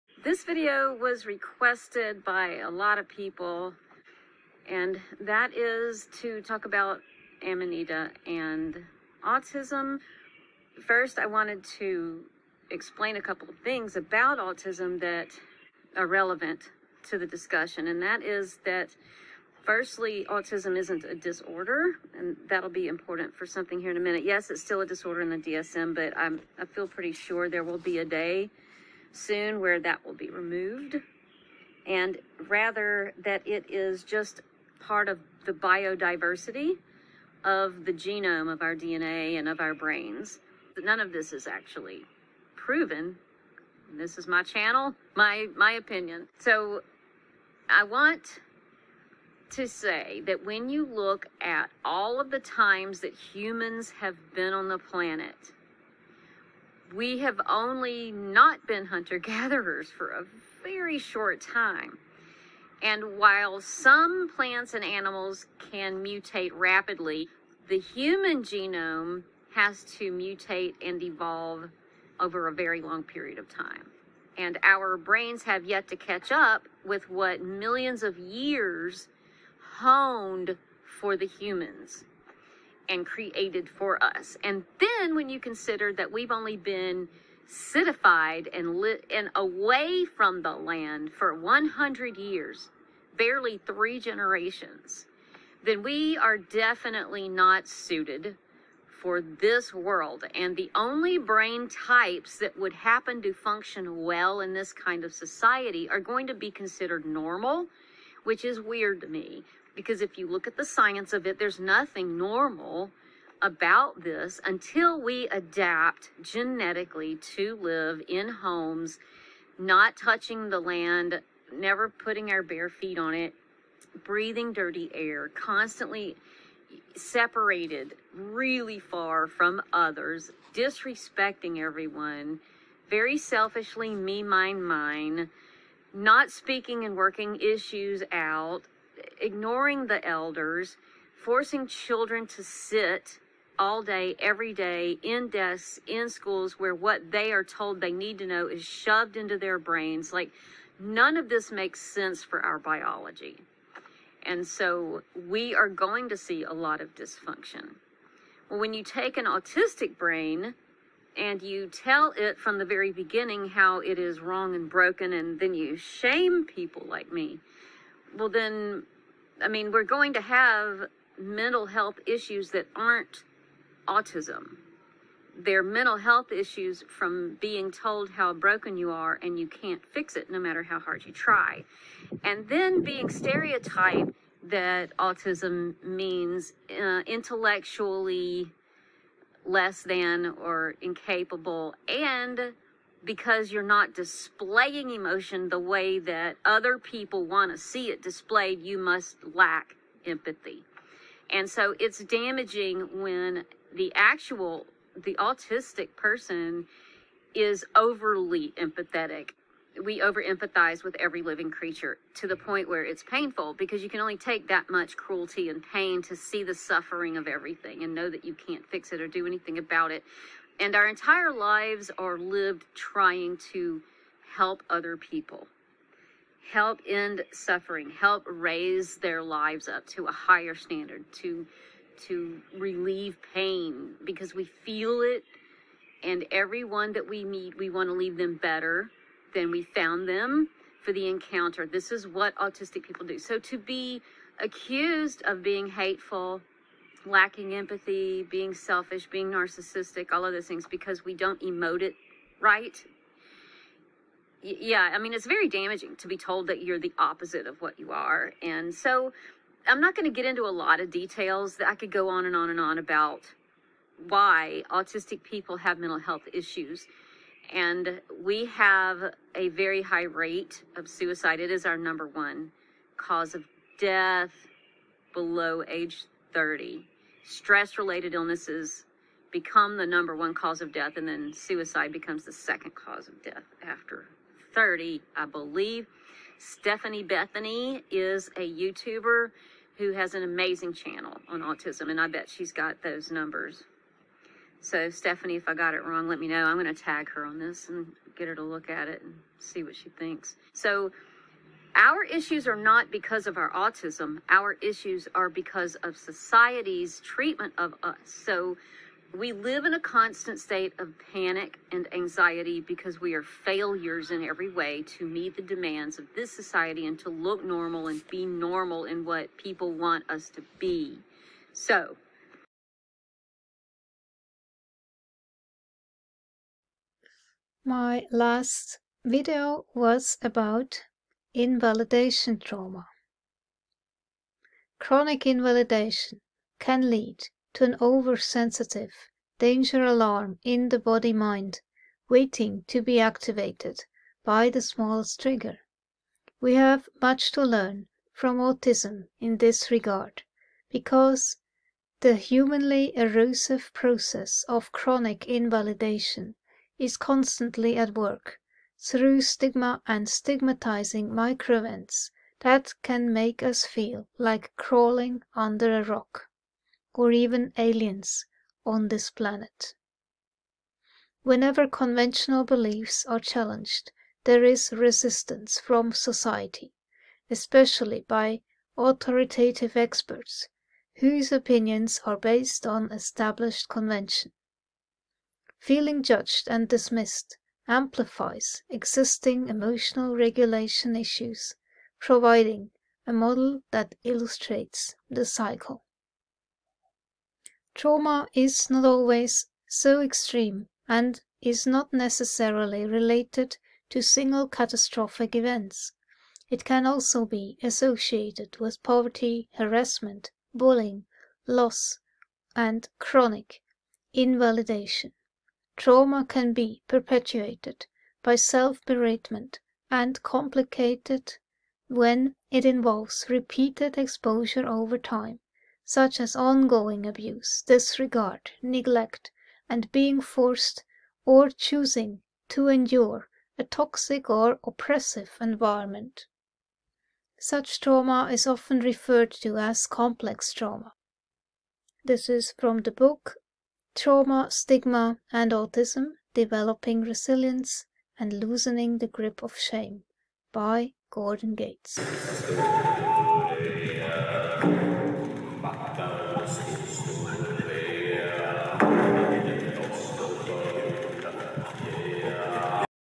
NAILED IT - WHY NEURODIVERGENT PEOPLE REALLY STRUGGLE
Have been saying this this in a very long time, it is THE ENVIRONMENT, stupid.
The societal cage, the intolerance of people being different.
THAT is the issue that causes stress and additional Invalidation trauma.
As a result, we self-isolate, we hide, so we are left alone.
But we have too much empathy, not a lack of.